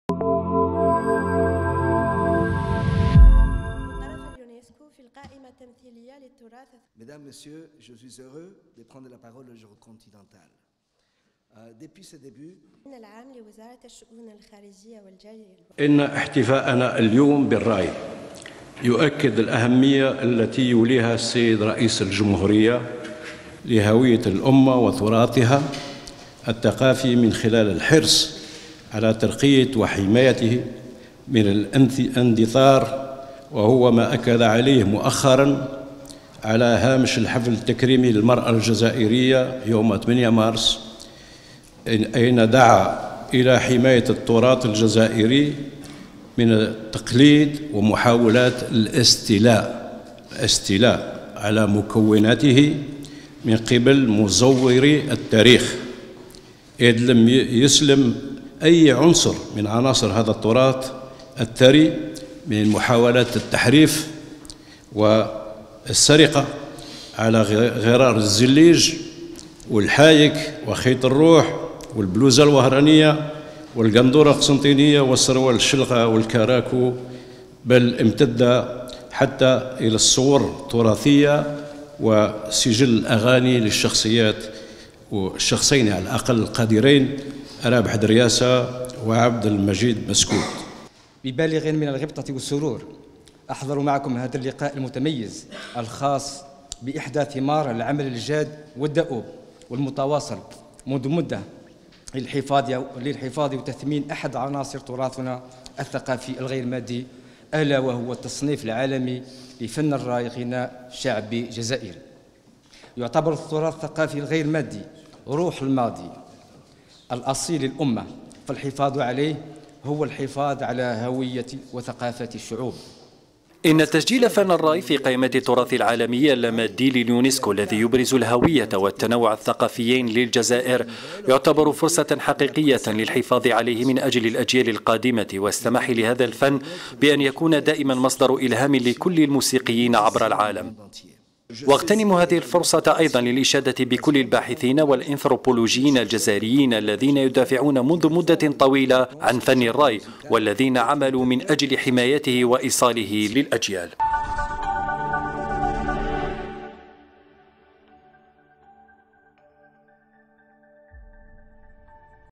الجزائر تتسلم شهادة إدراج أغنية "الراي" في قائمة التراث اللامادي للانسانية بمنظمة اليونسكو
الجزائر(وأج)- تسلم الأمين العام لوزارة الشؤون الخارجية والجالية الوطنية بالخارج، عمار بلاني، شهادة تسجيل "الراي"، غناء شعبي جزائري، في قائمة التراث العالمي غير المادي للإنسانية من طرف منظمة الامم المتحدة للعلوم والثقافة (يونسكو)، والتي سلمها له السفير المنسق المقيم لنظام الأمم المتحدة بالجزائر.